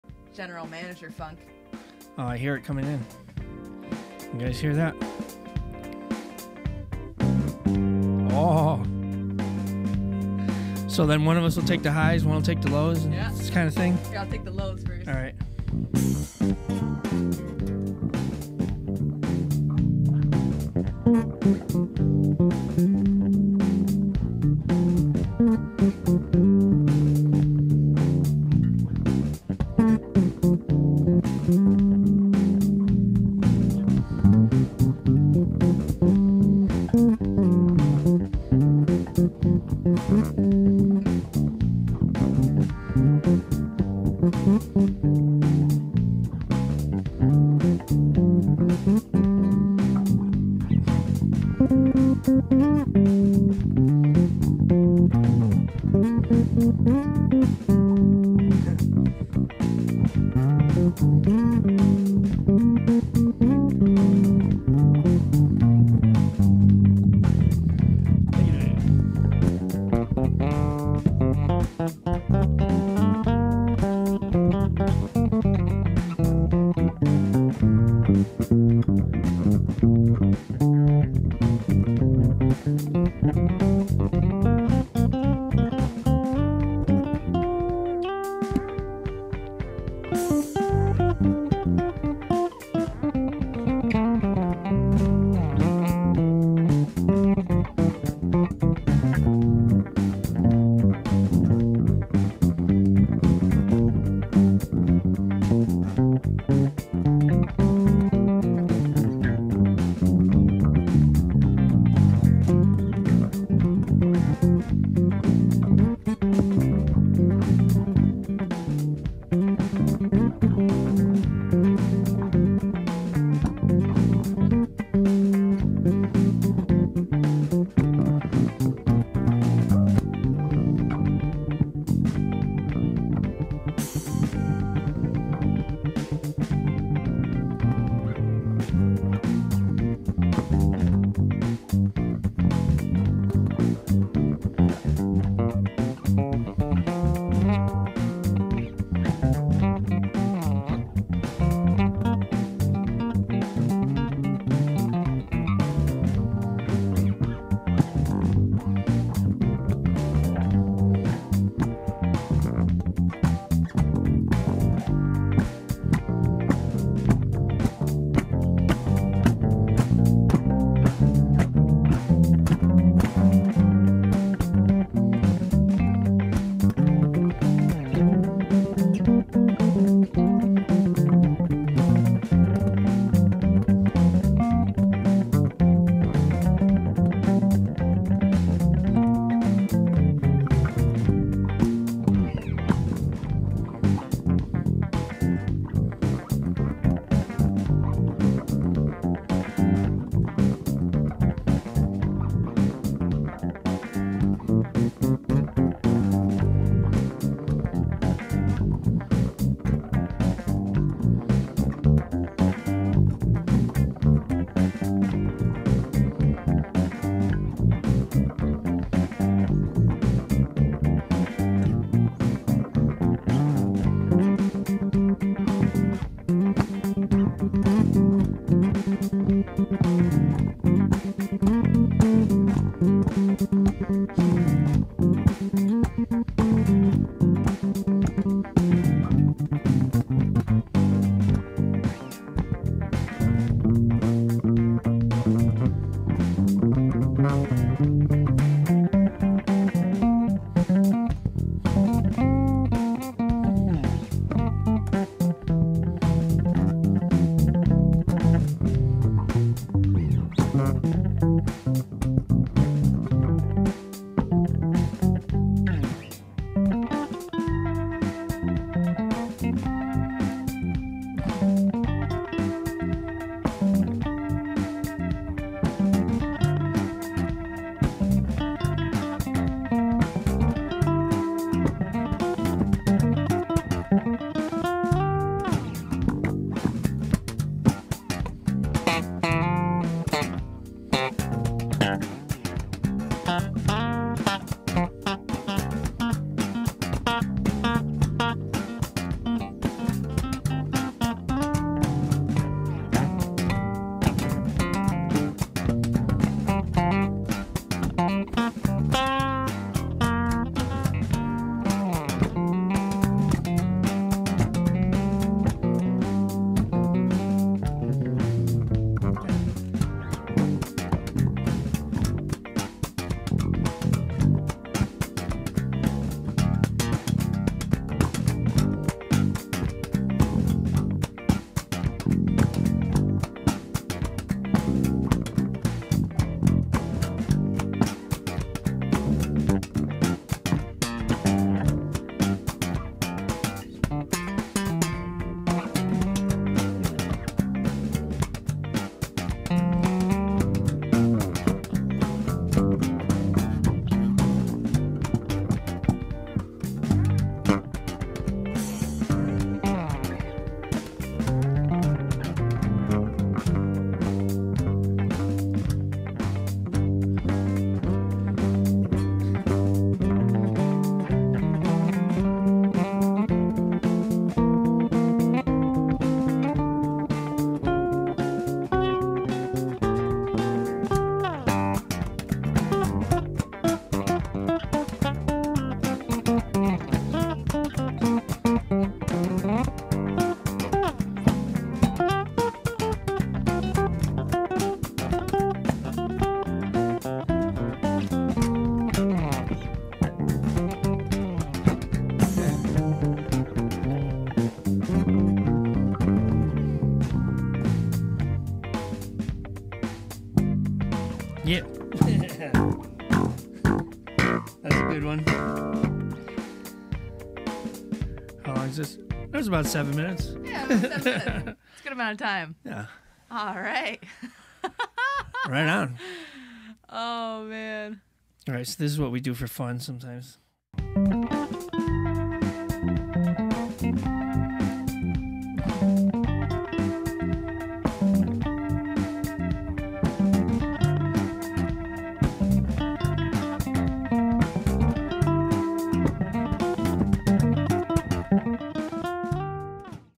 10 STRINGS OF BASS! Two Bass Guitar Jam w/ King Saison
Noelle and I jammed double bass style for this livestream show.  We had a ton of fun improvising over different backing tracks we found on youtube.  this is a clip of one of our jams from the March 25, 2023 show.

thank you to the creators of the backing track channels!